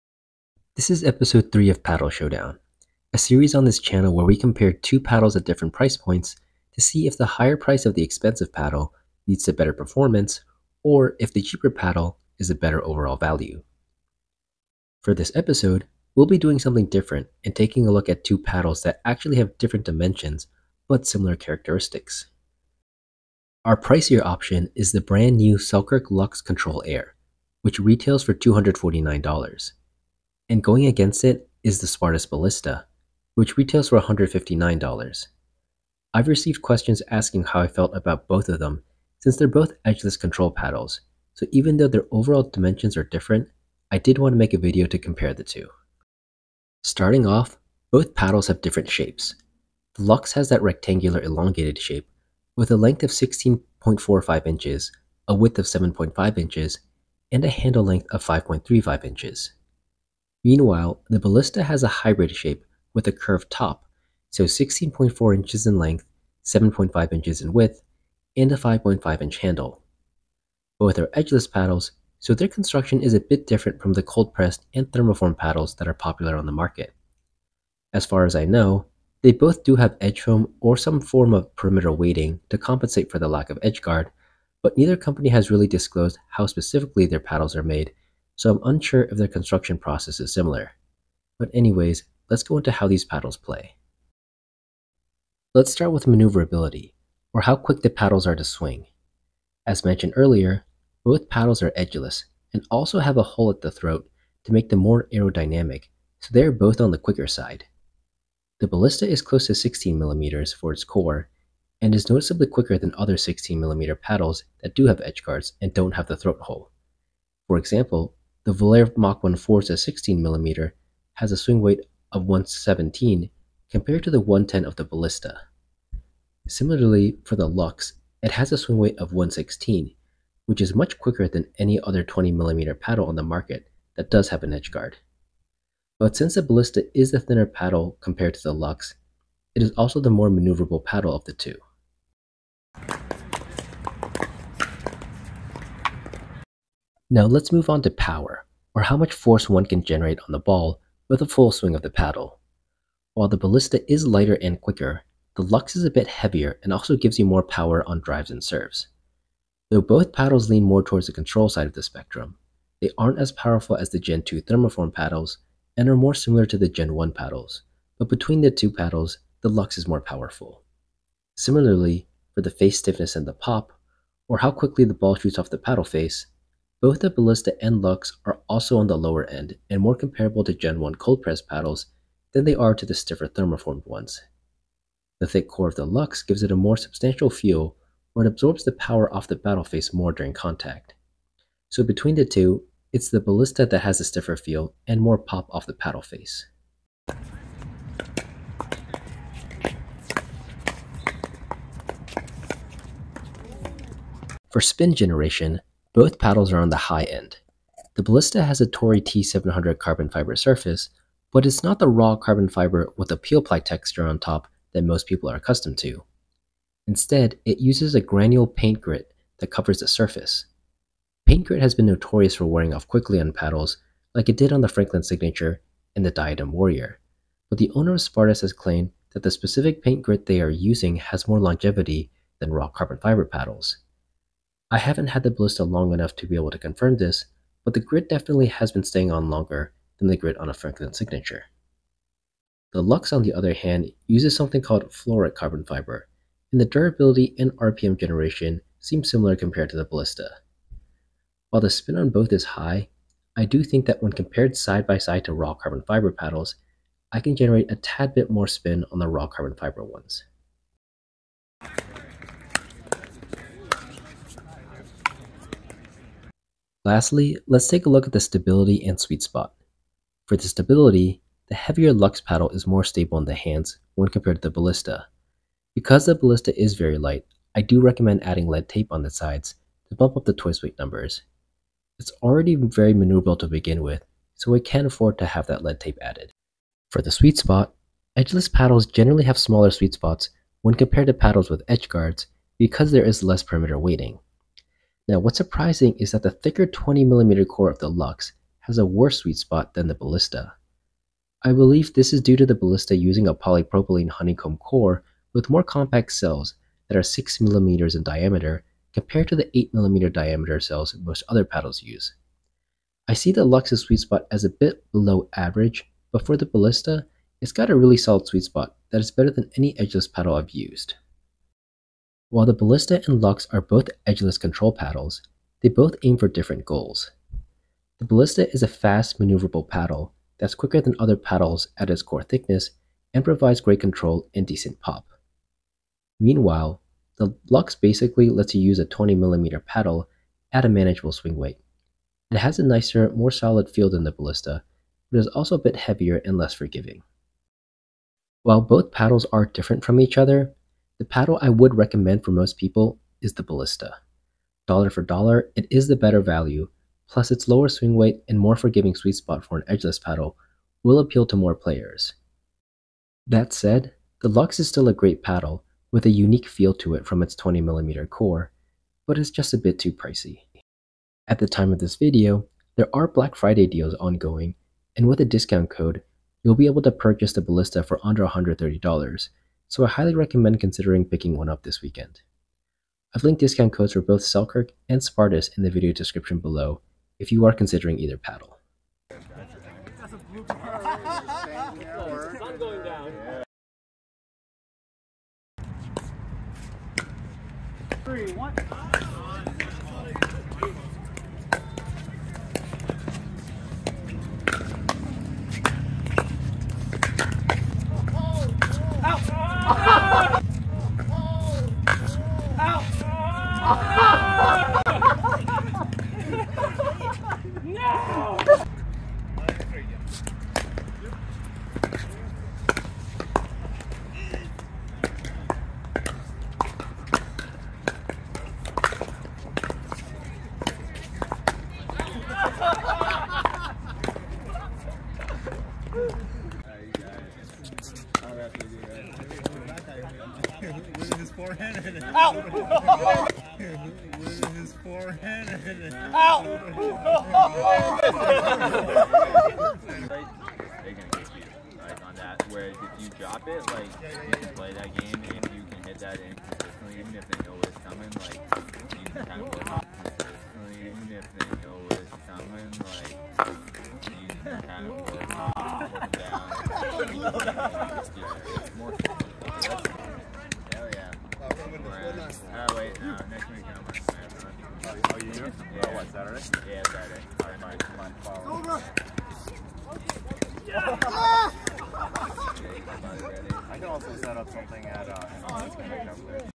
Paddle Showdown Ep 3: Spartus Ballista vs. Selkirk Luxx Control Air Invikta
Episode 3 of Paddle Showdown: Budget vs. Expensive Pickleball Paddles: Spartus Ballista vs. Selkirk Luxx Invikta

Use code ADV-DYANG for a gift card with your order from Selkirk